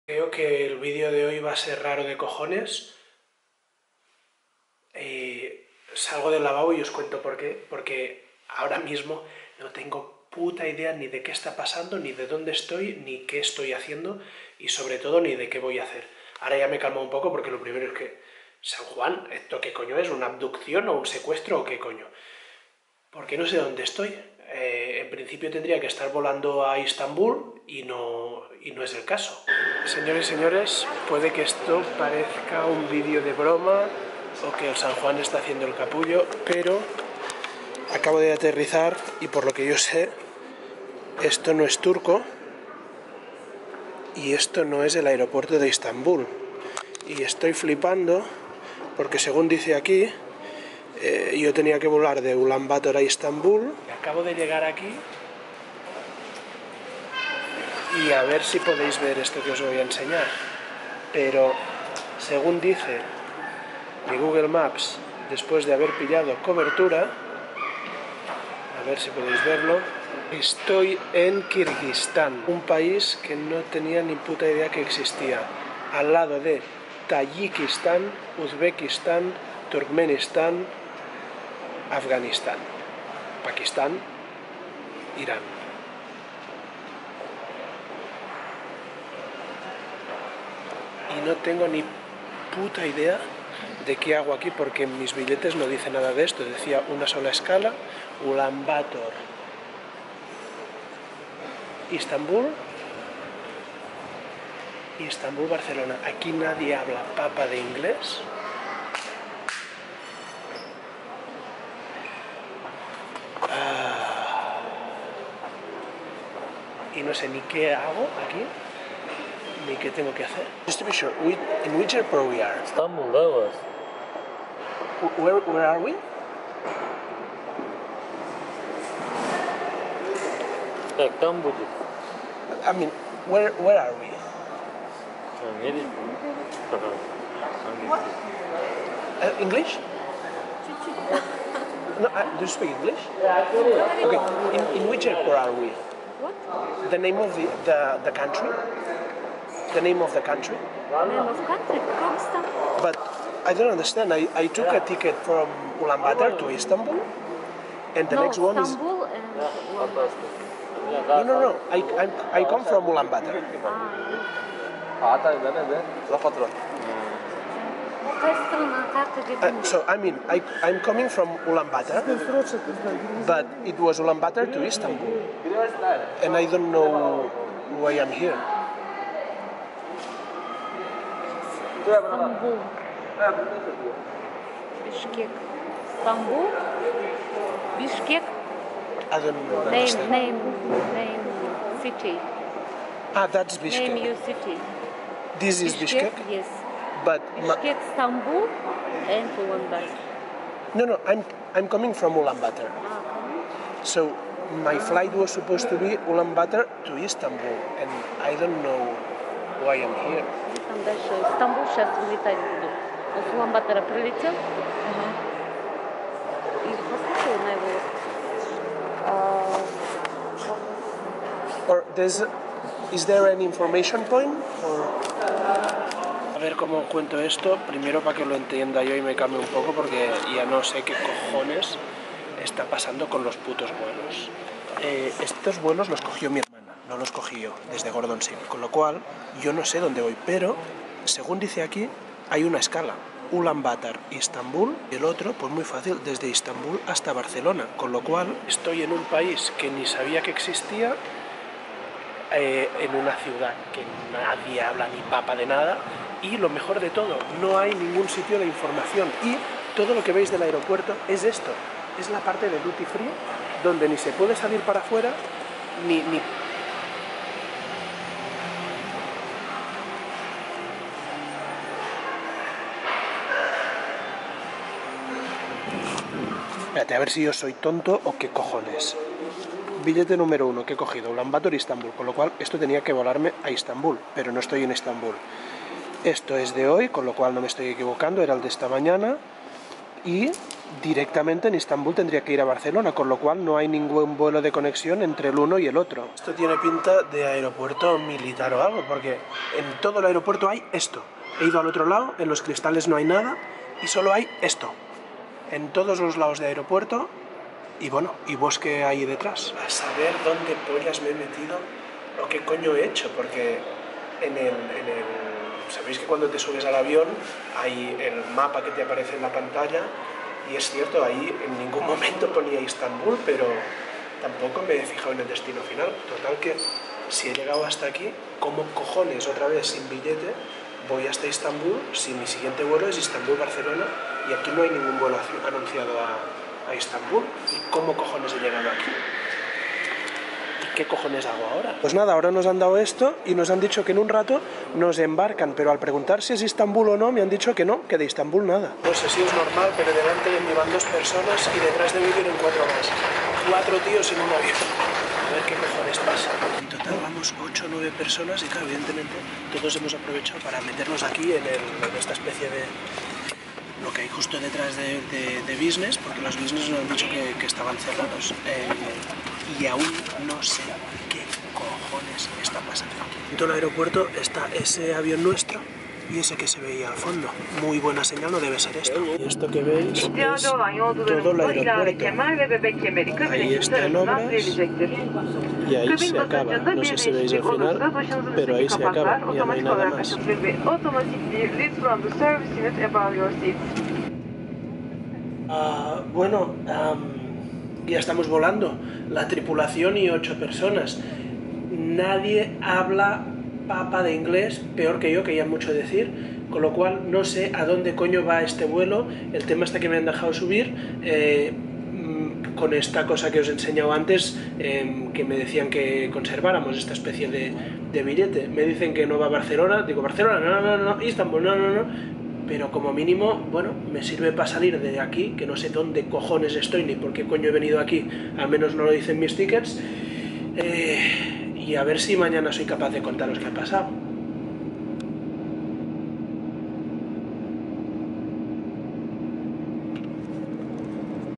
Perdido En Kirguistan (!!??)
Se supone que mi vuelo era: Mongola-Istanbul e Istanbul-Barcelona. Una única escala. Así lo dicen mis billetes y mi reserva. Pero he aparecido con 8 pasajeros más en KIRGUISTAN. Nadie habla papa de inglés. Y no hay ningún vuelo anunciado a Istanbul ni Barcelona.